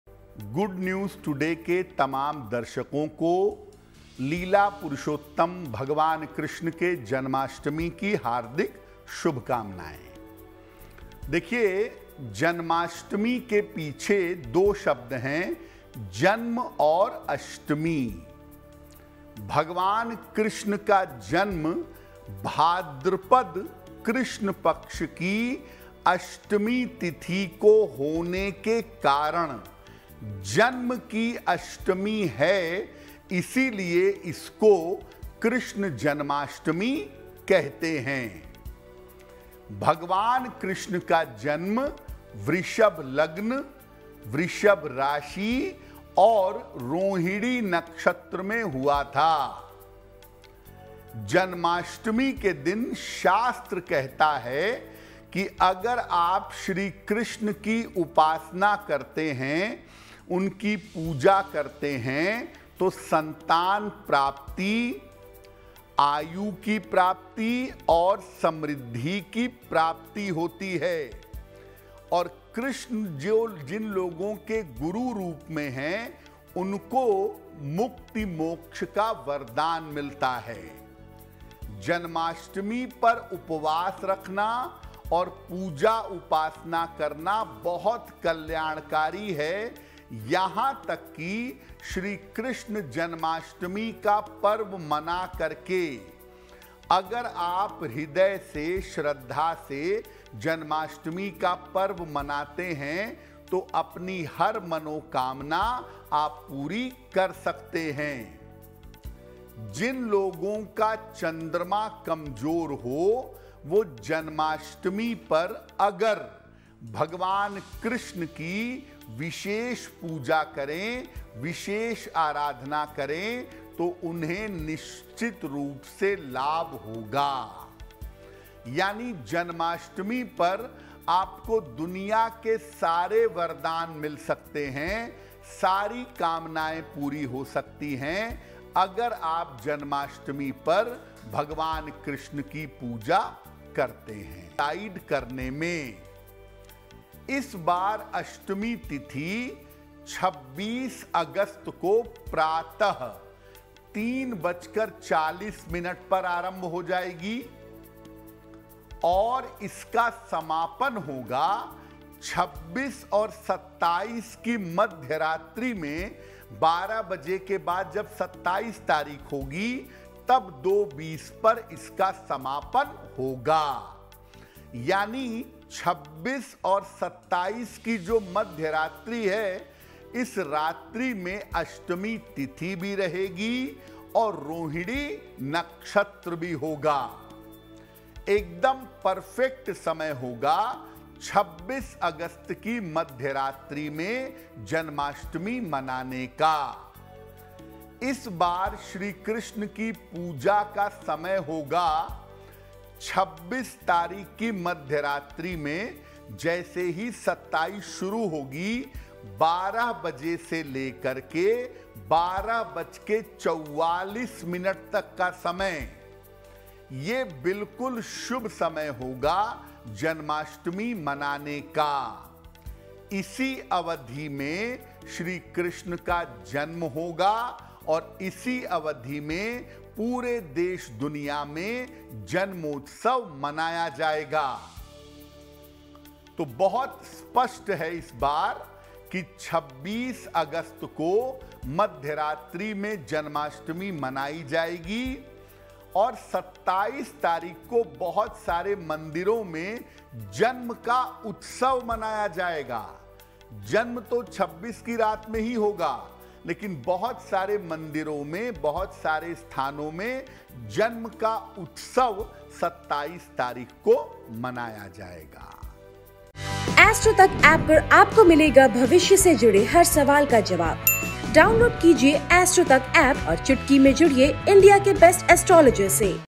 जन्माष्टमी पर श्रीकृष्ण की पूजा का ये है सबसे शुभ मुहूर्त | Shailendra Pandey | Astro Tak
Krishna Janmashtami 2024 : भगवान कृष्ण का जन्म भाद्रपद कृष्ण अष्टमी को होने के कारण इसको कृष्ण जन्माष्टमी कहते हैं. भगवान कृष्ण का जन्म अष्टमी तिथि को हुआ था, इसलिए जन्माष्टमी के निर्धारण में अष्टमी तिथि का बहुत ज्यादा ध्यान रखते हैं. जन्माष्टमी के दिन श्री कृष्ण के लिए छप्पन भोग बनाना चाहिए. ऐसा करने से श्रीकृष्ण प्रसन्न होंगे और आपको सुख समृद्धि की प्राप्ति होगी. श्रीकृष्ण जन्माष्टमी का पर्व मना कर हर मनोकामना पूरी की जा सकती है और विशेष पूजा से लाभ पा सकते हैं...ज्योतिर्विद शैलेंद्र पांडेय जी से जानते हैं कि, जन्माष्टमी पर श्रीकृष्ण की पूजा का ये है सबसे शुभ मुहूर्त ...